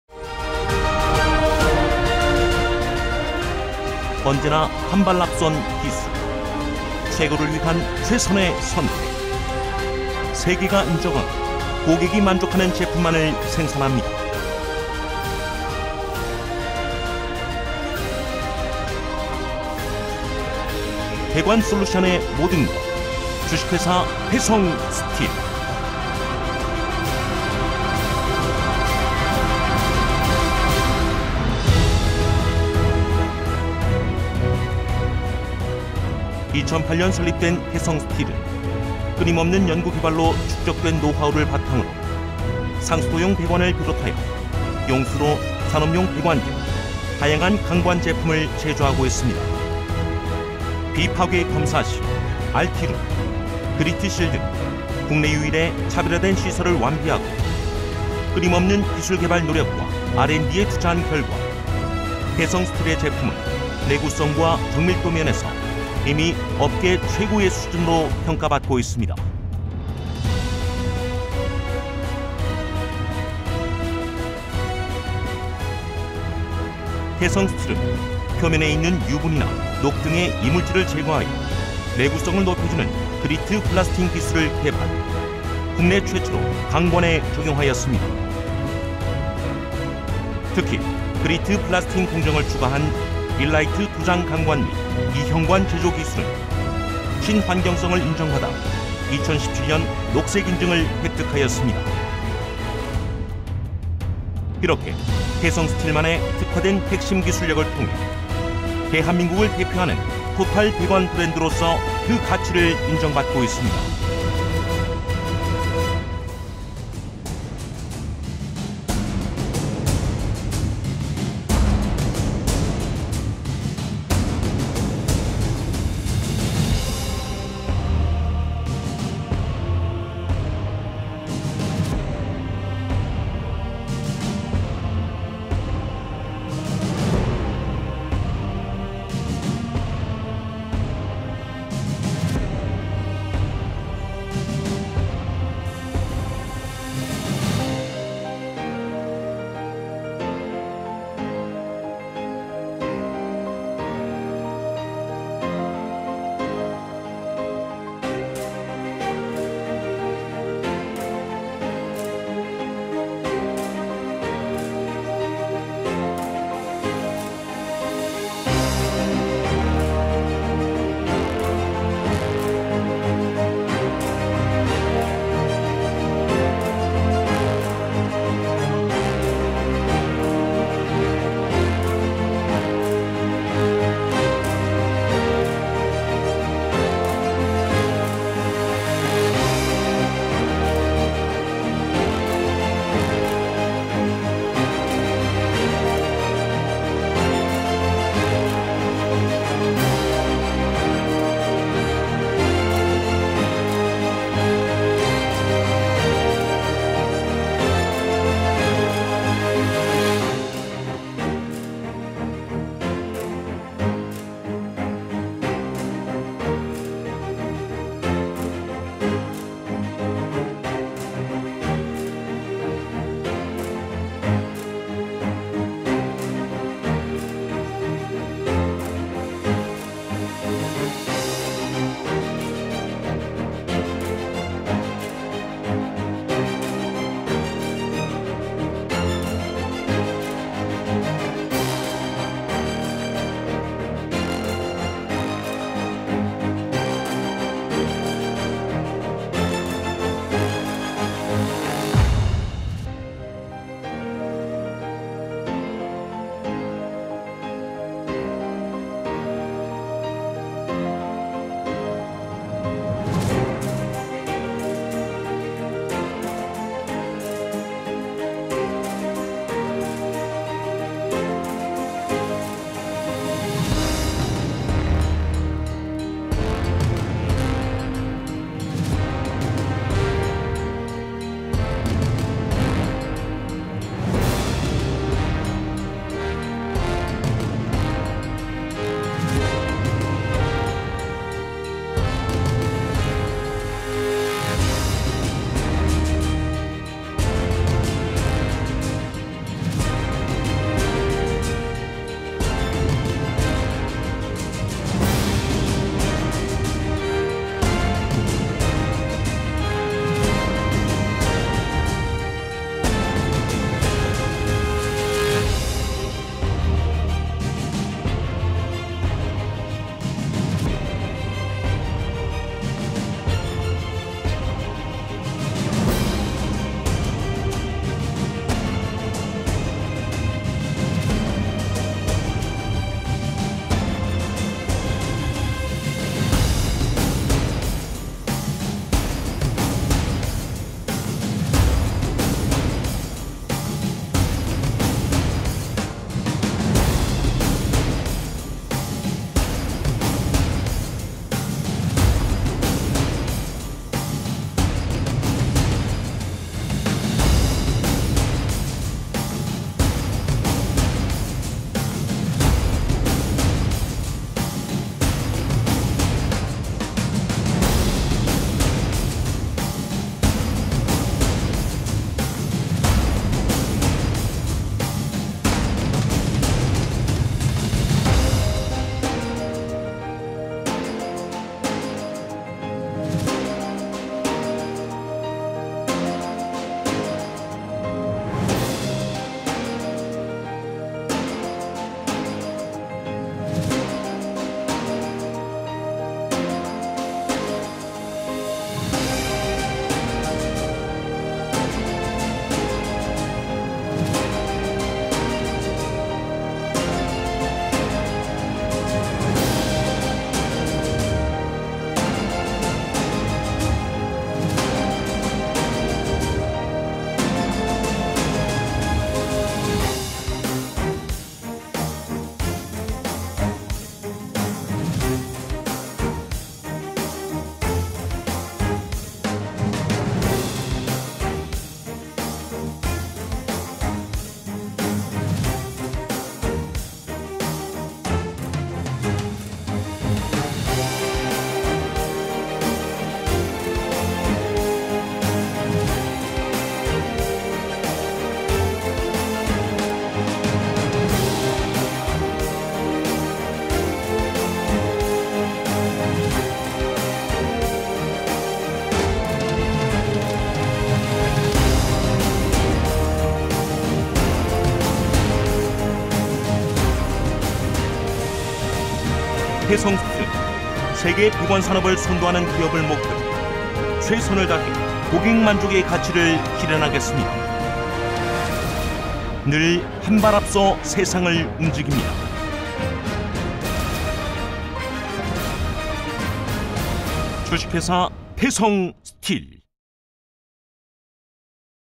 [태성스틸] 홍보 영상 (폴리에틸렌 3층 피복강관 및 이형관 공정 소개)
충북 음성군에 소재한 피복강관 업체인 태성스틸입니다.

(주)태성스틸은 '최고의 품질관 신뢰'를 기업철학으로 2008년 법인을 설립하여, 상수도용 도복장 강관 및 이형관을 생산하는 종합 배관업체로 성장하였습니다.

당사 제품에 대해 문의 사항이 있으시면 메일 또는 사무실로 연락바랍니다.

■ 태성스틸의 대표 설비의 구성 및 효과

1) 기존 전처리 쇼트 블라스트 외 추가 전처리 그리트 블라스트 실시

2) 그리트볼과 와이어 재질로 이루어짐

3) 고압의 에어로 분사시켜 내면의 산화물, PE유분 등 이물질 제거

4) 표면조도향상에 따른 부착력 증대 (강관수명증대)

■ 분말 3층 기능성 일라이트 에폭시 도장강관 및 이형관 특장점

1) 그리트 블라스팅 공정을 추가하여 조도 향상

2) 조도향상에 따른 도료의 밀착성과 부착력 강화로 강관의 내구성 증대

3) 일라이트를 함유한 "무용제 타입, 비스페놀-F 계열" 친환경 도료 사용으로 인한 안전성 확보

4) 강관 외면 코팅에 3층 피복을 적용함으로써 내구성과 내충격성을 향상

5) 경과시간 30분 이내로 시공 및 유지보수 용이

6) 고도 기술인 조달우수제품, 녹색기술, 녹색기술제품 취득